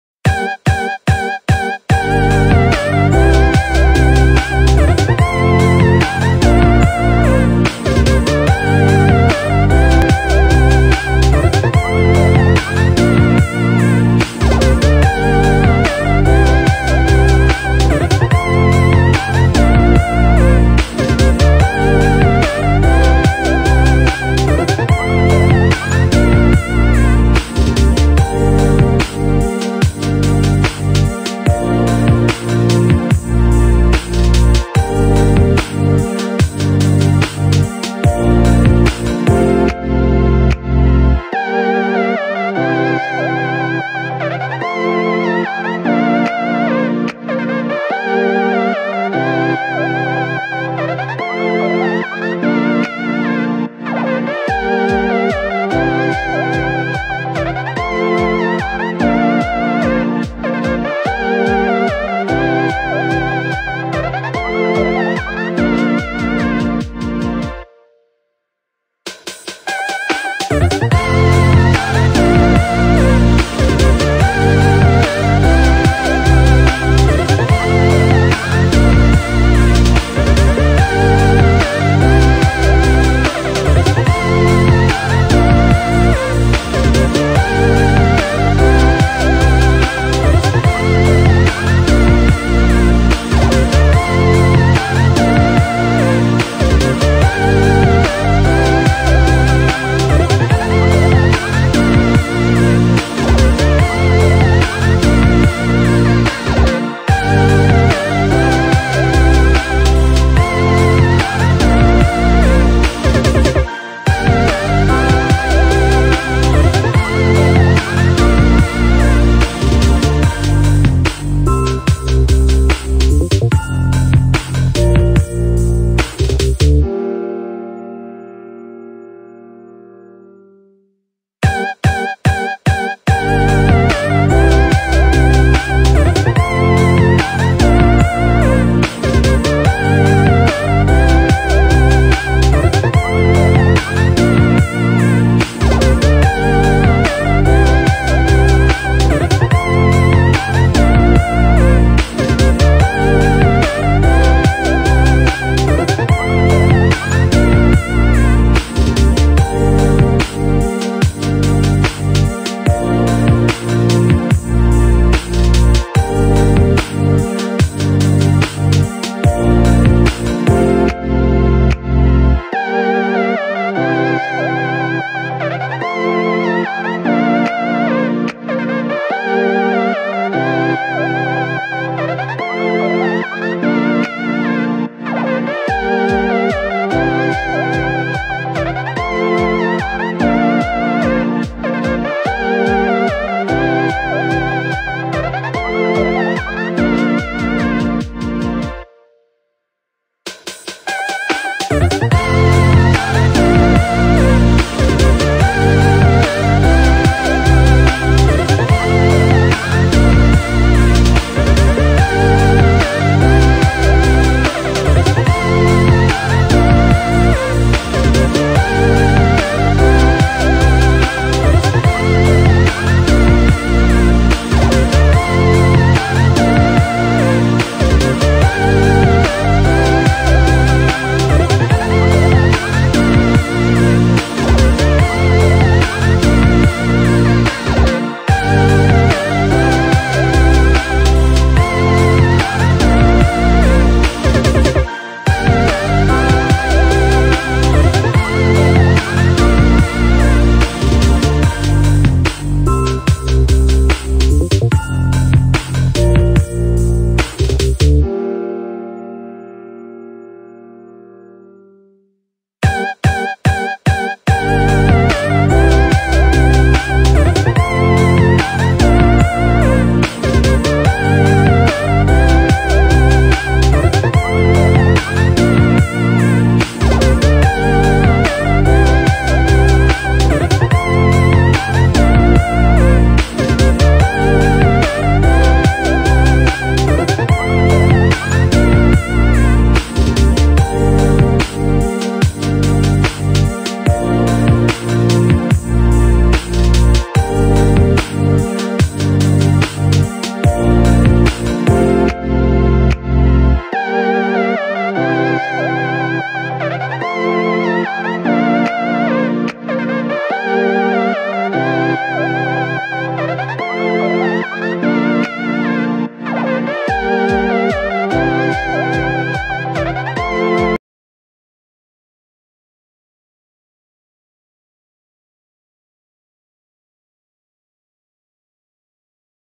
Teknik Lari ditempat dan teknik melatih kekuatan lengan secara berkala Training Wushu Sanda Kids
Teknik Lari ditempat, gerakan ini dapat dilakukan dirumah diusahakan diruangan terbuka dan untuk menguatkan lengan anak usia dini. Semua gerakan ini sangat mudah dilakukan untuk pemula.